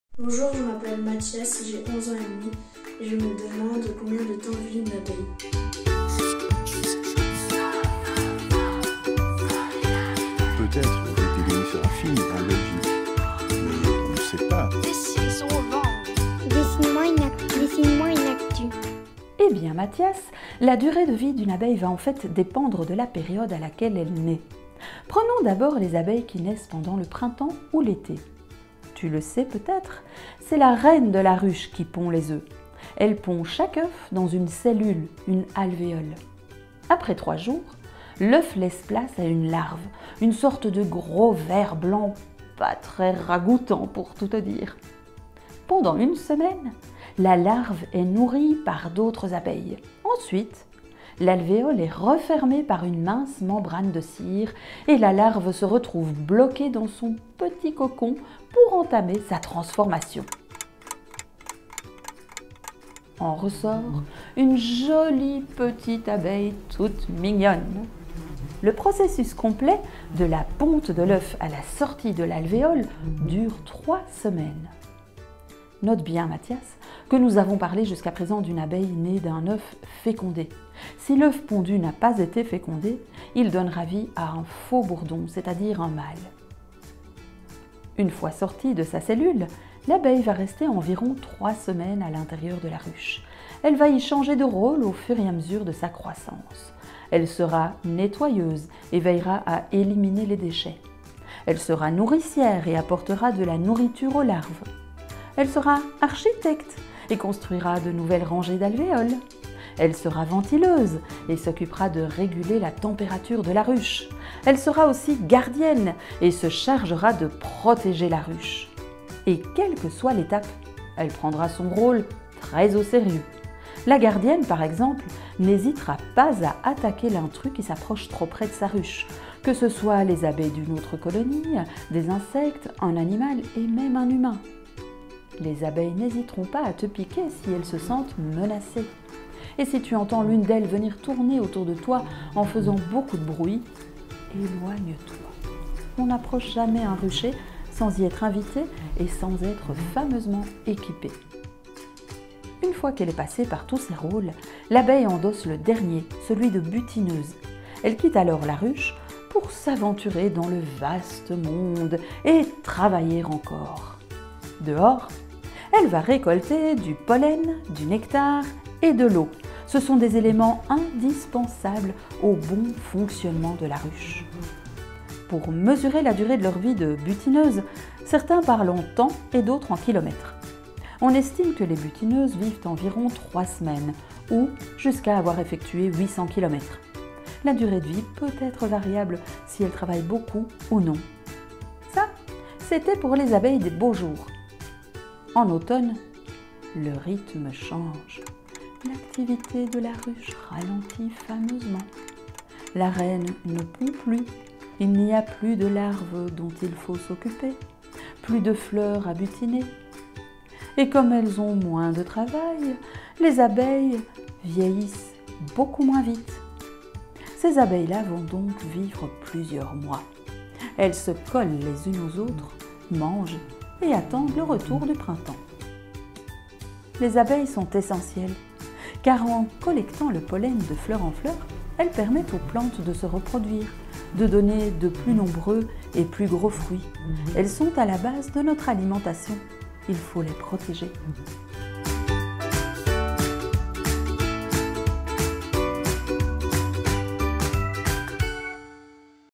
Combien de temps vit une abeille? Dessine-moi une actu (45)
Essentielles pour la biodiversité et à la base de notre alimentation, les abeilles sont fascinantes. Mathias se demande combien de temps dure leur vie. La réponse en vidéo.